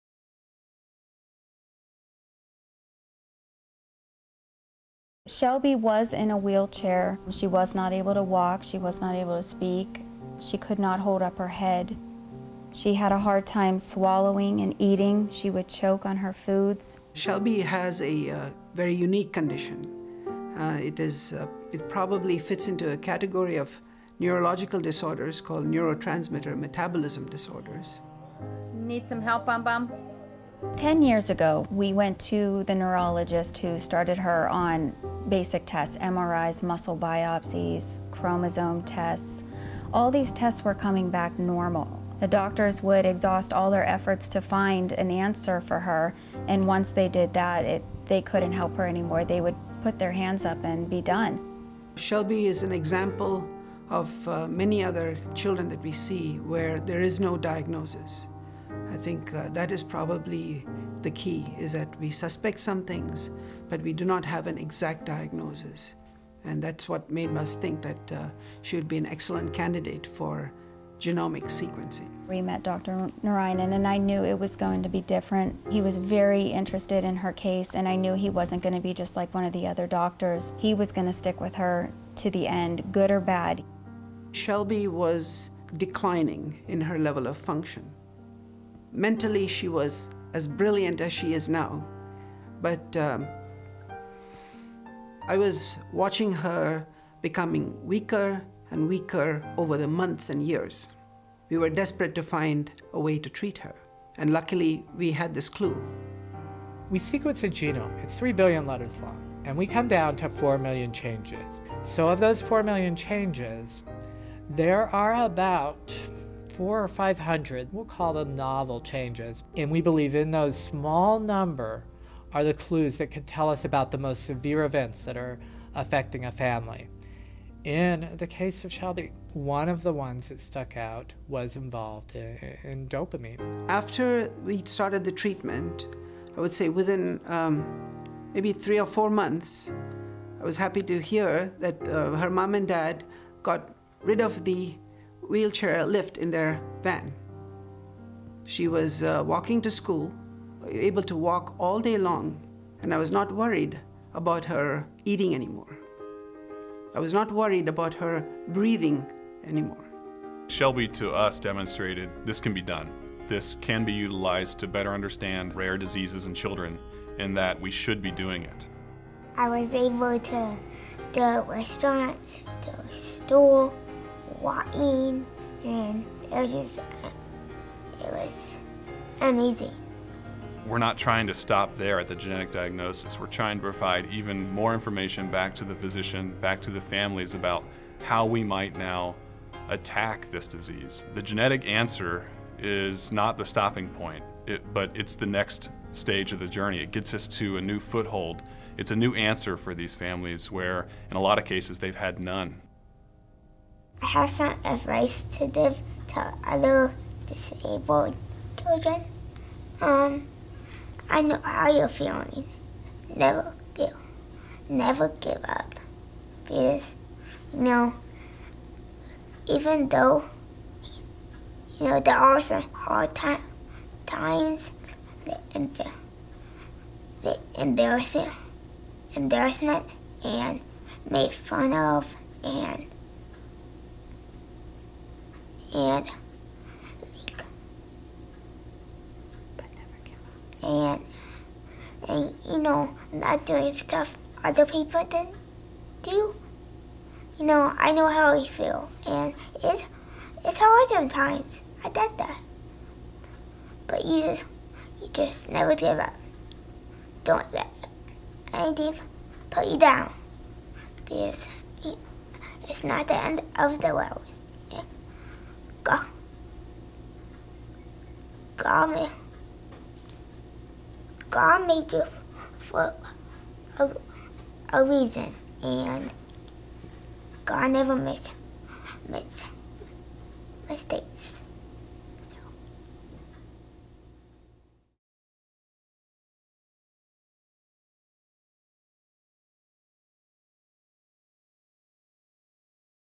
Personalized Medicine Helps Girl Walk Again
This is the story of Shelby, a 12-year-old Phoenix girl who was wheelchair-bound for nearly a decade due to a rare disease of unknown cause. She had difficulty walking, talking, holding her head up, swallowing, and even breathing... but the key word in that description of Shelby is "had". Scientists at the non-profit Arizona research institute TGen sequenced Shelby's genome; something they couldn't have done in a reasonable amount of time or cost just five years ago. By sifting through the unique alterations in her data they were able to provide clues to the genetic basis of her rare disorder and empower her physician to prescribe a medicine based on this new information that resulted in changes to Shelby that were nothing short of a miracle. Today, she can talk and walk, even run, and is a 7th-Grade honors student.